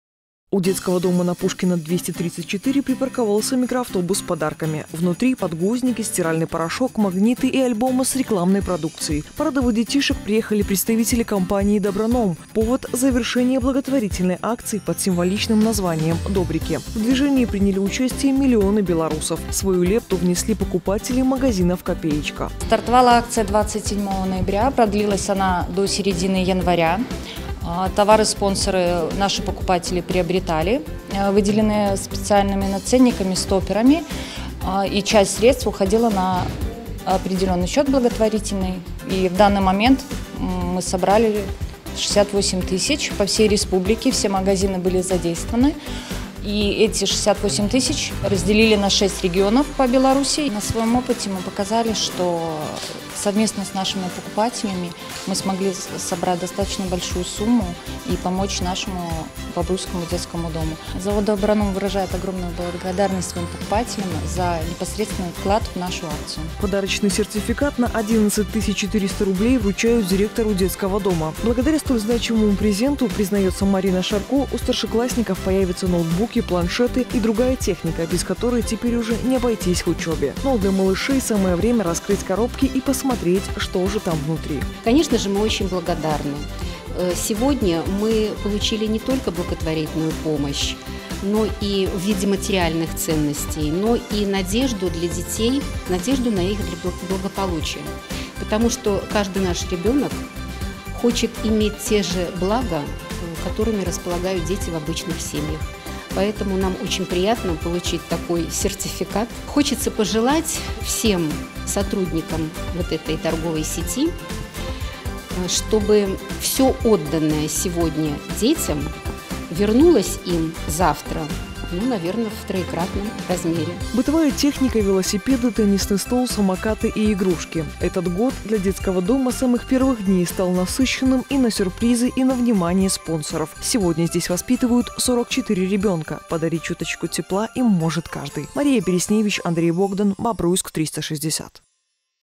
Компания "Доброном" сделала подарок детдому.
Подарков много не бывает! В детский дом Бобруйска заглянули представители торговой сети «Доброном». Кроме нужных презентов благотворители привезли и сертификат на 11 400 рублей. О доброй акции и лепте покупателей – далее.